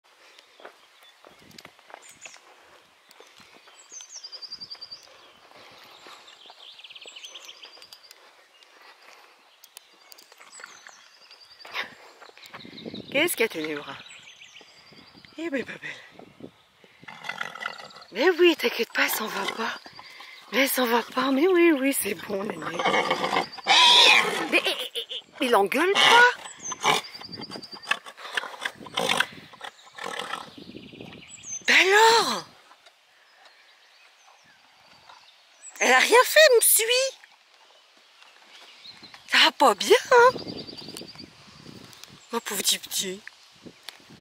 2020 04 27 pre c
Dipti, qui n'a fait donc que me suivre depuis le début...se fait sacrément rappeler à l'ordre par sa mère !
Je pense que ce qui a agacé Ténébra, c'est que, malgré les appels qu'elle faisait en revenant vers nous, Dipti l'a ignorée !
En tout cas rappel efficace parce que Dipti ne m'a plus suivie quand je suis repartie !